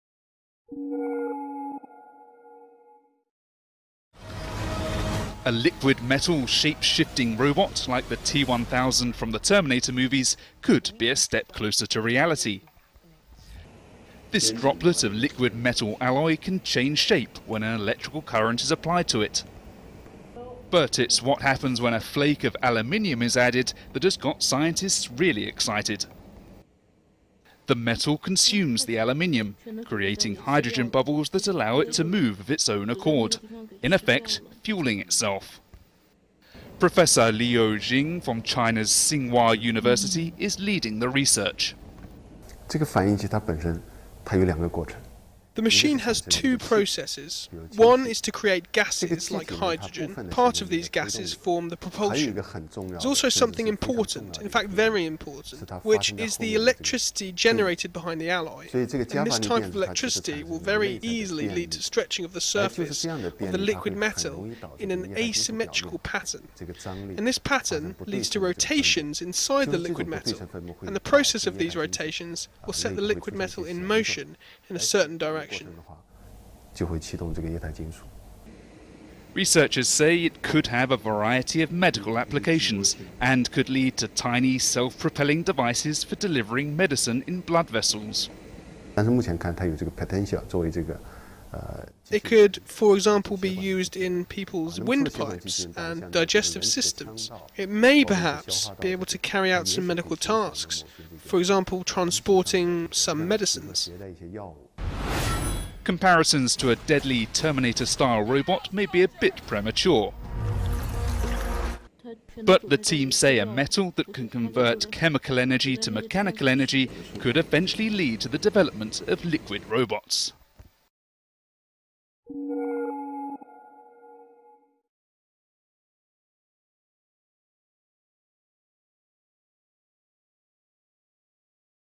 Liquid metal discovery paves way for shape-shifting robots
Scientists inChinaproduce droplets of liquid metal that can change shape and move of their own accord, bringing researchers a step closer to creating a \'Terminator-style\' shape-shifting robot.

This video was originally published on 10 April 2015